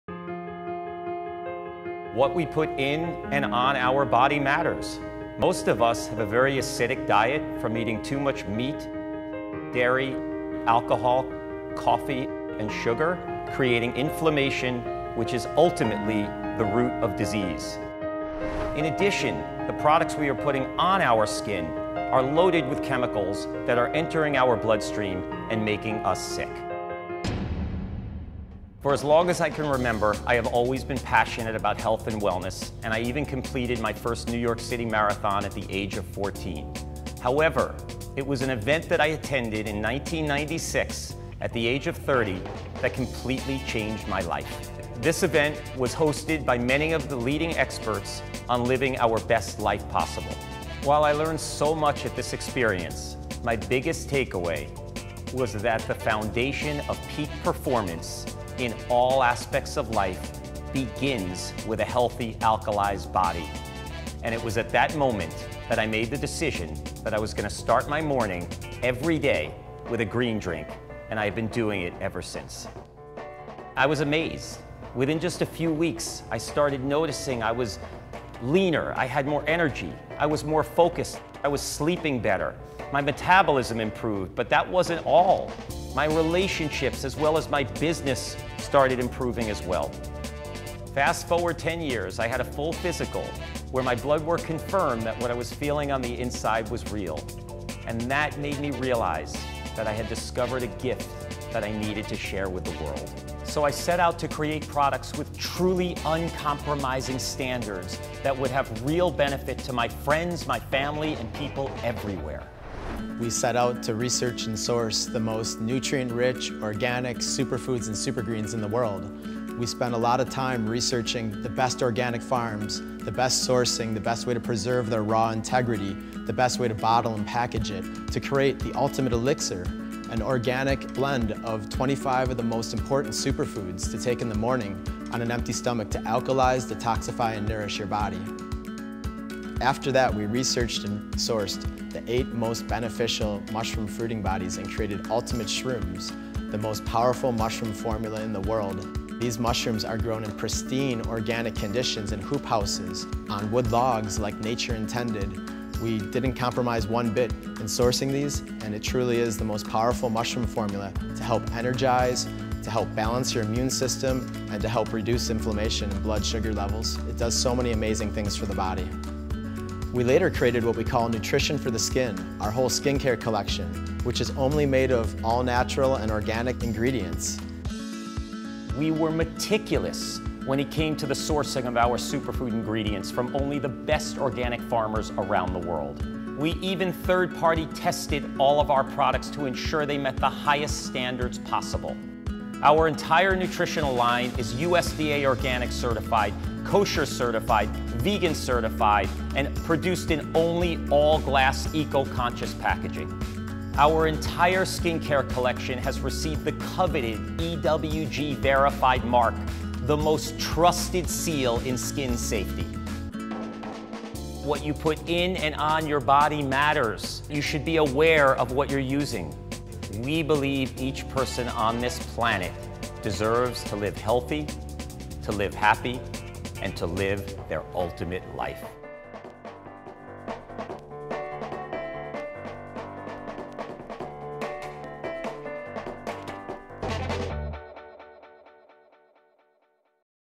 Live Ultimate - Best Supplements Immune System
Hi everyone,

Live Ultimate is such a fantastic company. Their products are literally second to none, and this is why they're featured in leading magazines and shows. I'm using their Ultimate Elixir (25 organic superfoods crushed into a powered to take daily) and their phenomenal Ultimate Shrooms (the best organic mushrooms) and it simply changed my life. I feel better, I sleep better, I'm focused all day. We created this video for them as a part of a campaign which included many others.

Check it out, and if you like any of the products - you can get them for a discounted price I get as well:

Enjoy and Live your Ultimate Life! :)